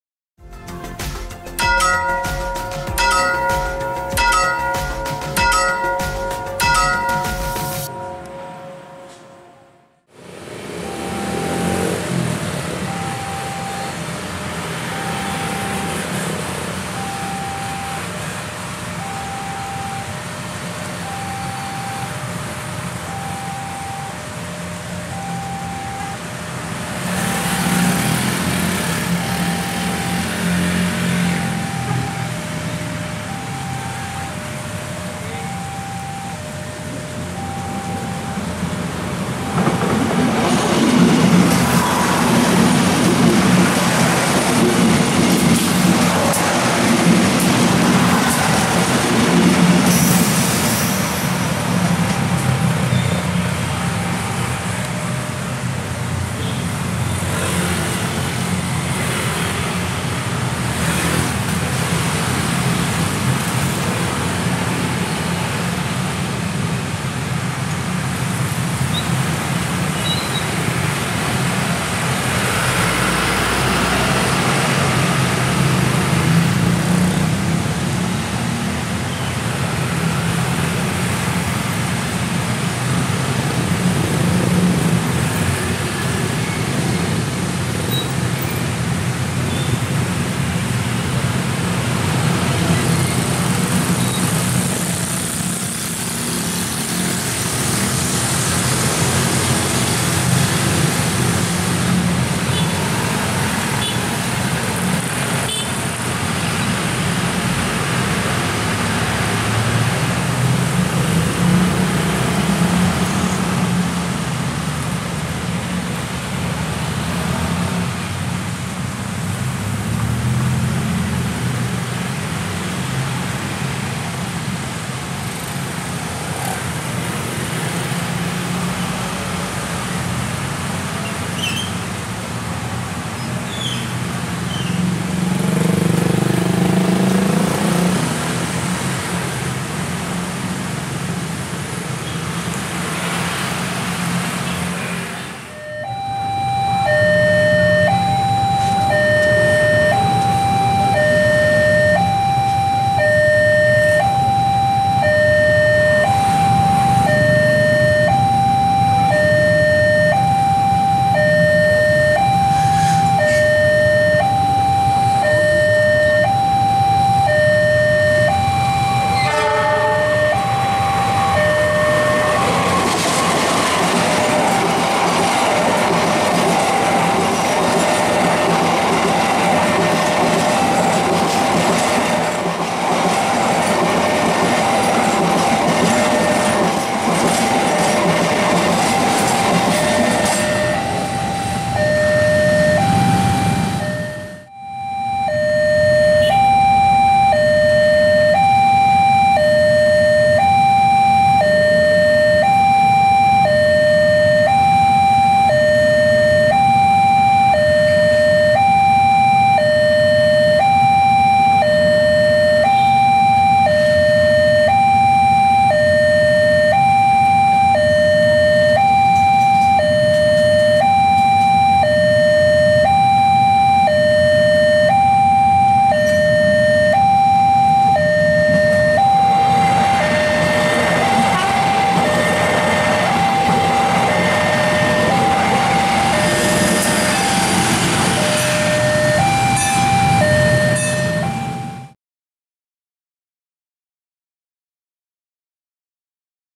KA BIAS (Bandara Internasional Adi Soemarmo) Megal-Megol Memasuki Jalur 3
Jalur 3 stasiun Klaten adalah tempat KA. BIAS berhenti untuk menurunkan penumpang dari arah Solo dan menunggu penumpang untuk keberangkatan tujuan Solo.
Ternyata dari jalur 1 menuju jalur 3, jalur kereta terlihat berbelok-belok sehingga KA. BIAS tampak berjalan 'megal-megol' layaknya seorang perempuan yang berjalan dengan menggerakkan pinggulnya ke kiri dan ke kanan, hehehe..
Sayangnya, dari JPL 279 ke lokasi wesel cukup jauh, sehingga gambar tidak terlalu jernih / jelas. Mungkin lain kali bisa saya coba mendekat ke lokasi, atau mendekat ke sebelah timur Depo KRL.